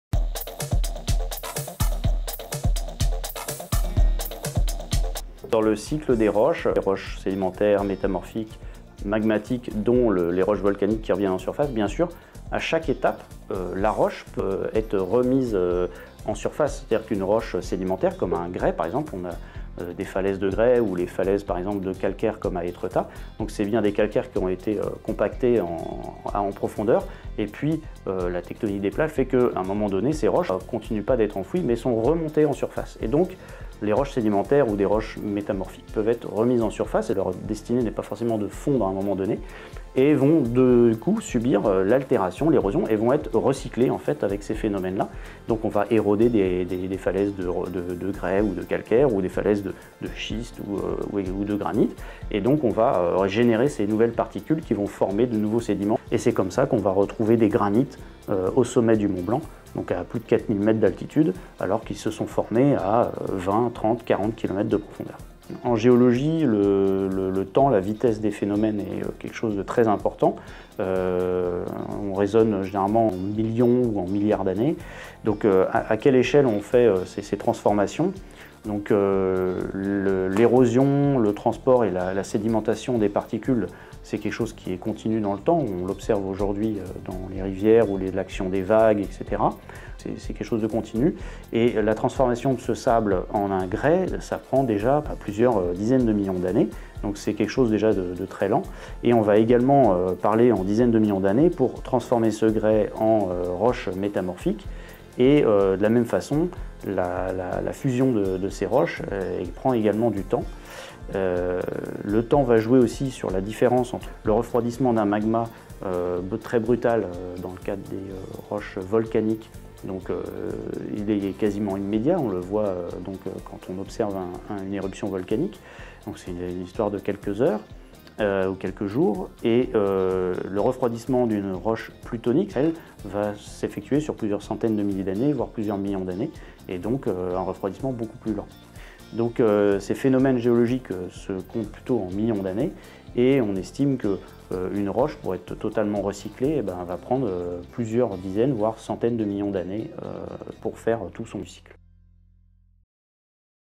Le cycle des roches : épisode 4, la durée du cycle
Combien de temps les roches mettent-elles pour réaliser leur cycle complet ?

Découvrez-le dans le dernier épisode de notre série sur la formation des roches.